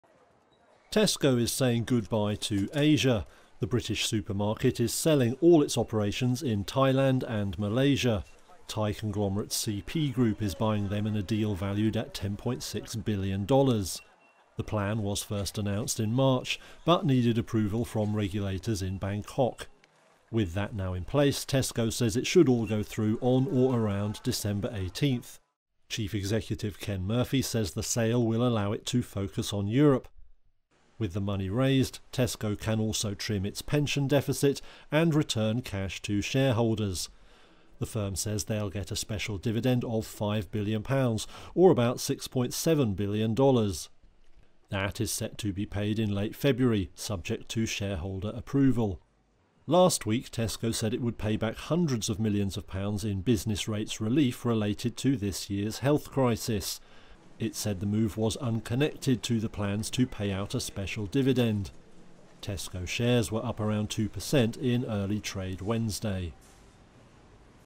Tesco sells Asia business for $10.6 billion
Britain’s biggest retailer Tesco expects to complete the $10.6 billion sale of its Asian businesses to CP Group on Dec. 18, paving the way for a return of $6.7 billion to shareholders.